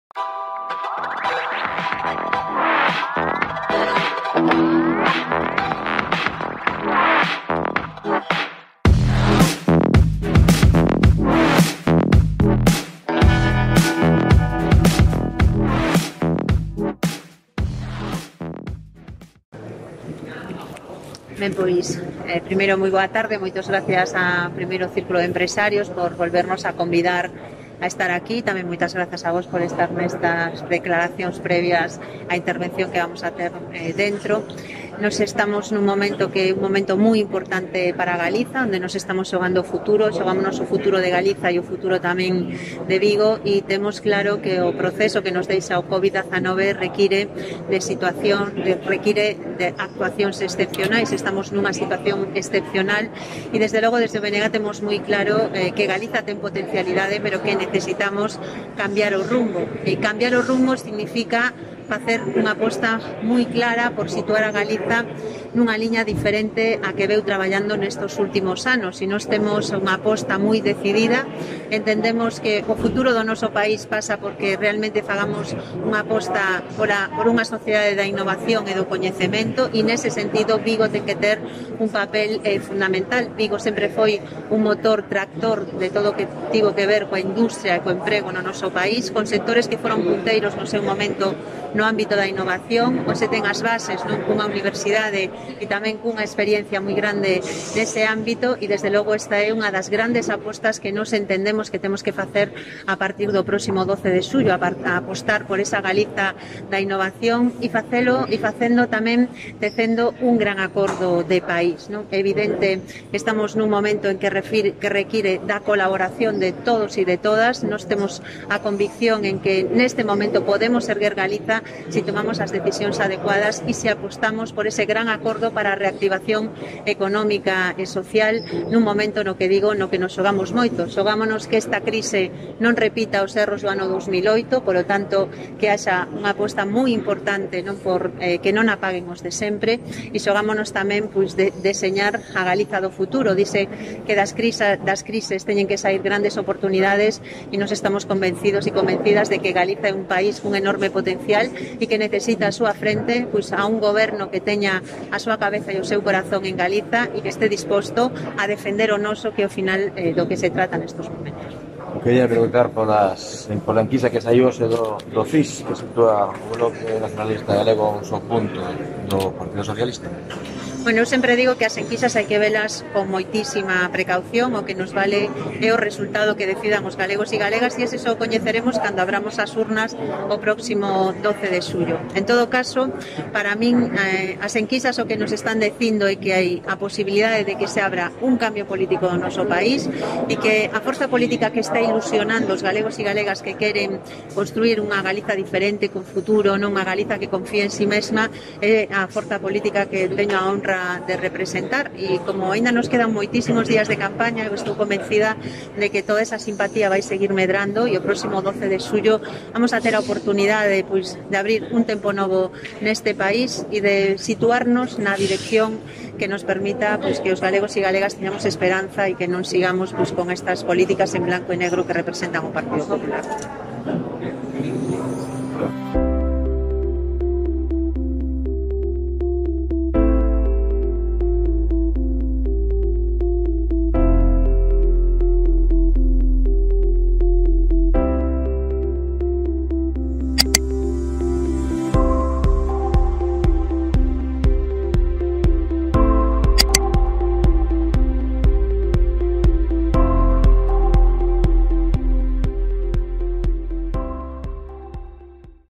Pontón propón un gran acordo de país para a reactivación económica e social de Galiza ante o Círculo
Ana Pontón propón un gran acordo de país para a reactivación económica e social de Galiza ante o Círculo de Empresarios en Vigo